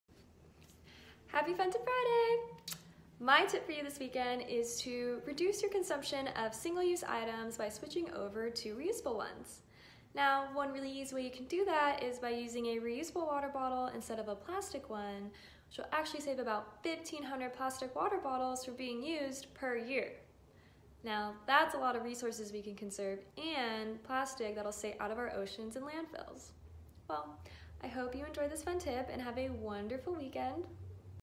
Fun-Tip Friday: Reduce Single-Use
Reduce your consumption of single-use plastic by making the easy switch to a re-usable water bottle!